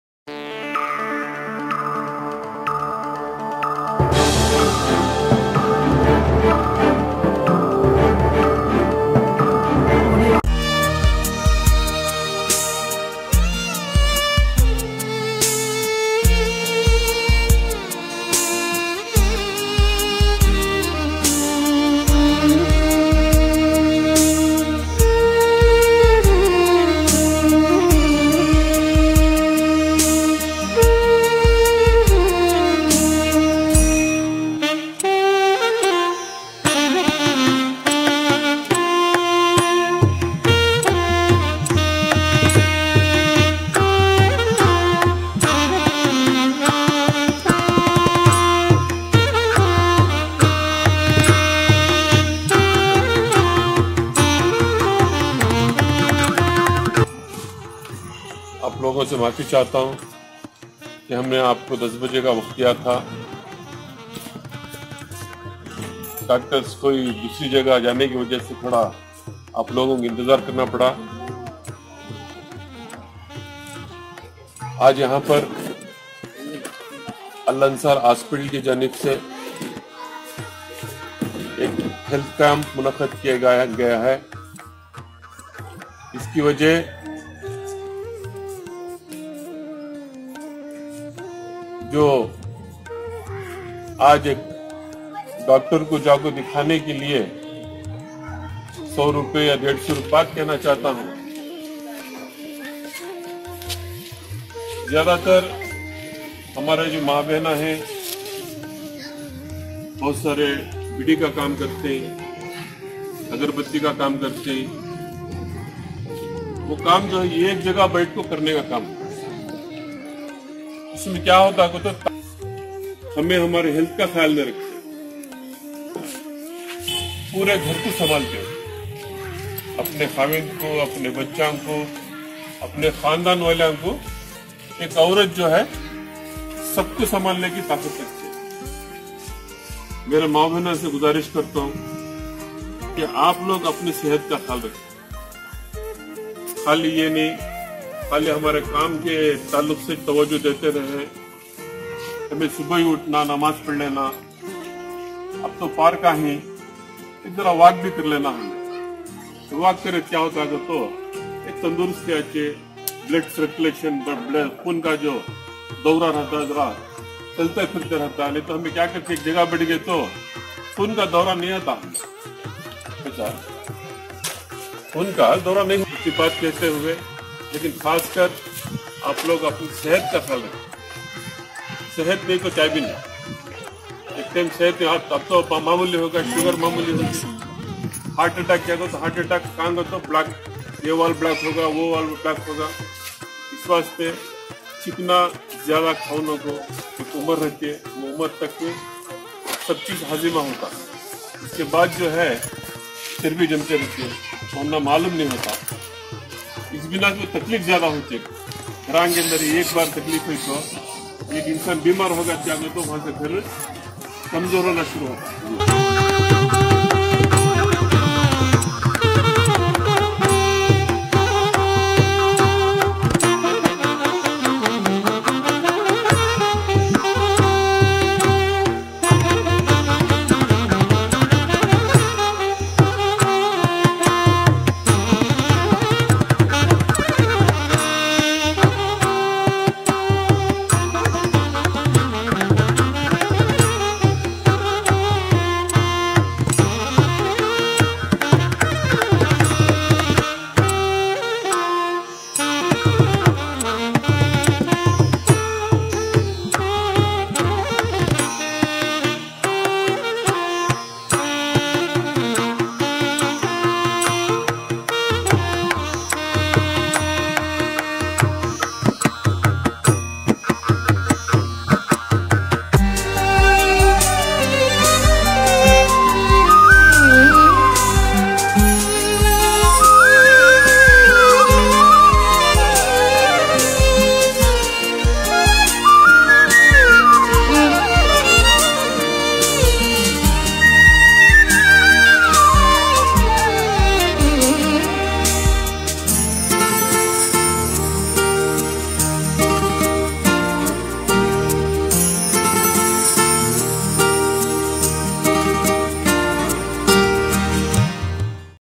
Mysore: Free health Camp held at Govt urdu school kesare in association with Alansar Hospital by Soc
Mysore: Free health Camp held at Govt urdu school kesare in association with Alansar Hospital by Social worker Ilyas Ahmed @ Babu
Reported by Kausar News 24x7 Channel India